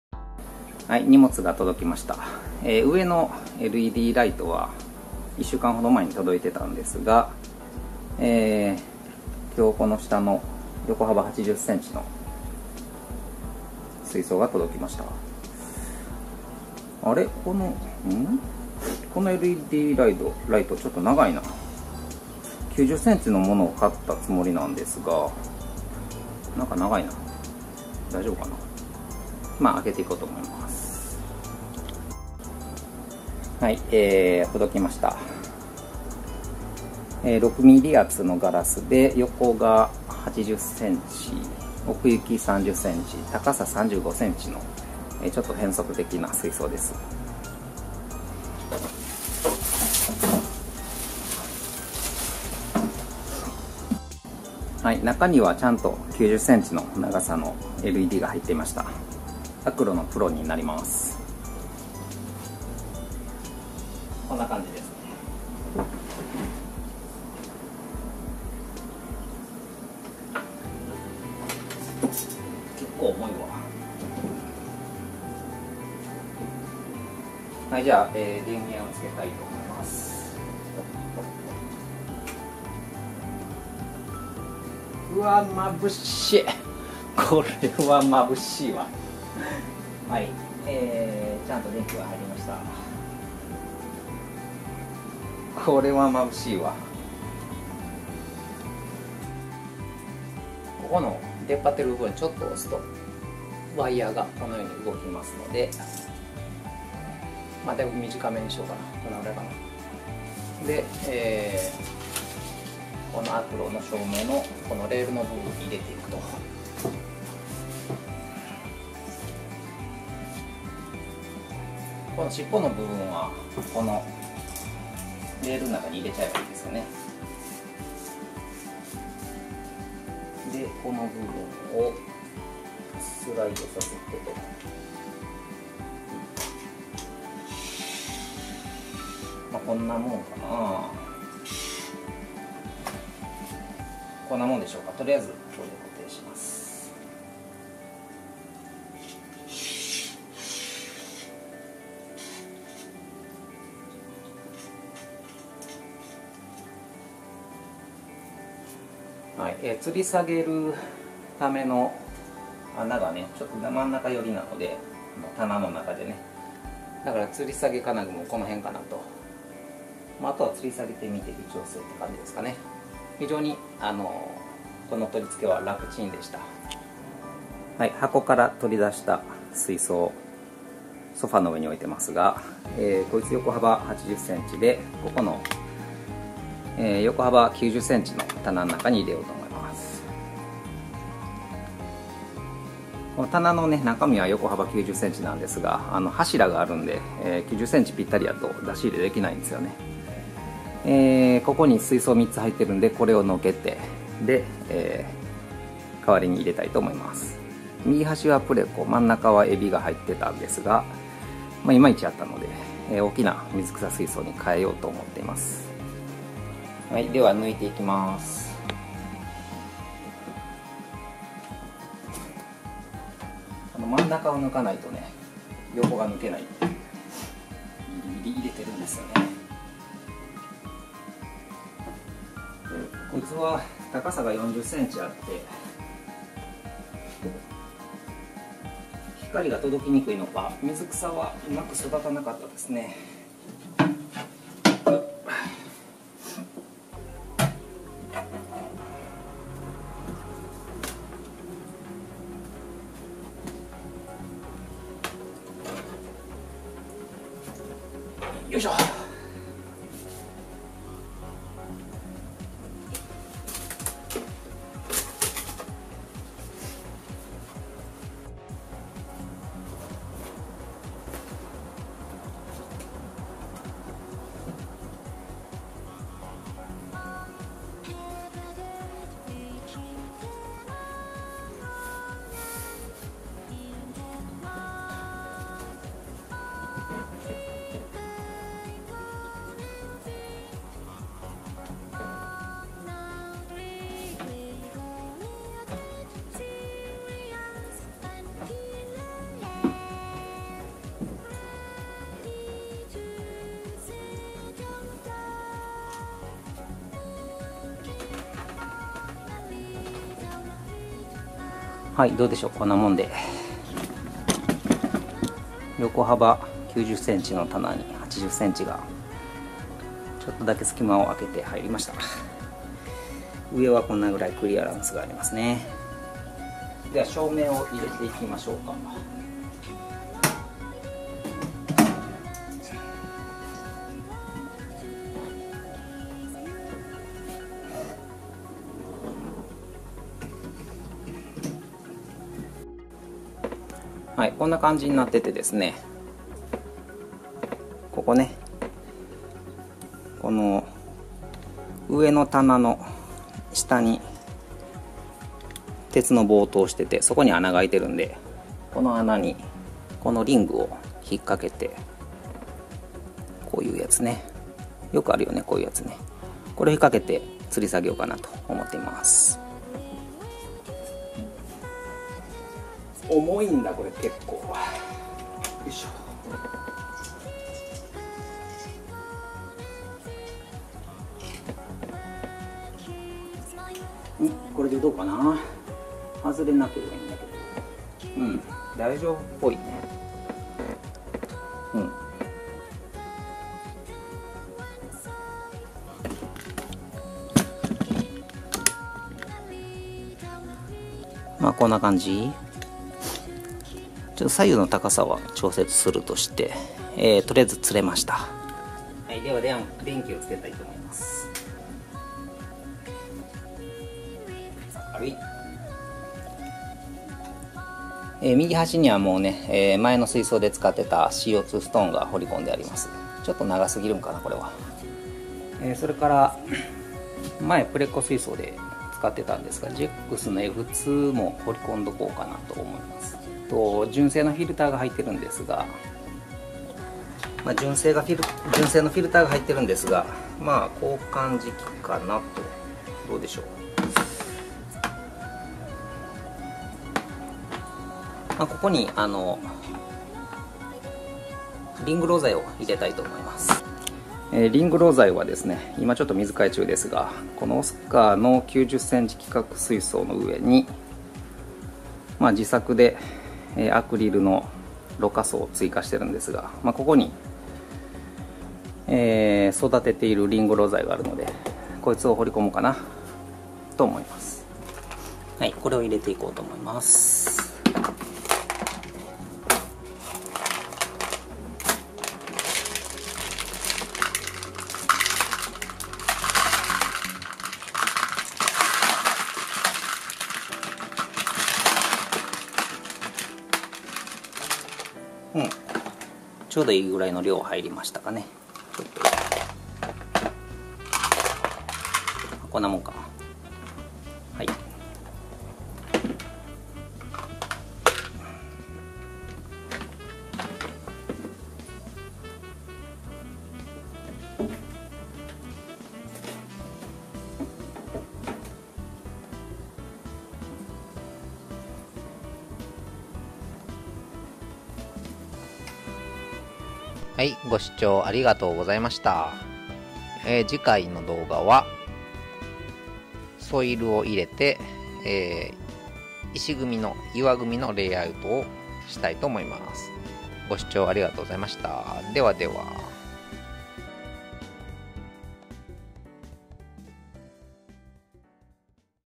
【水槽285】チャームのLED ”トライアングル GROW Pro 900” 買ってみた【オーダーメイド水槽立上げ①】
プレココーポレーションに特注水槽（横幅800mm × 奥行300mm × 高さ350mm）を作ってもらい、チャームのLED ”トライアングル GROW Pro 900” で照明したいと思います（まぶしい）。

今回は水槽とライトの開封、ライトの吊り下げ、GEXのコーナーフィルターF2の濾過槽にリングろ材を入れる所まで。

次回はソイルを入れて石組み～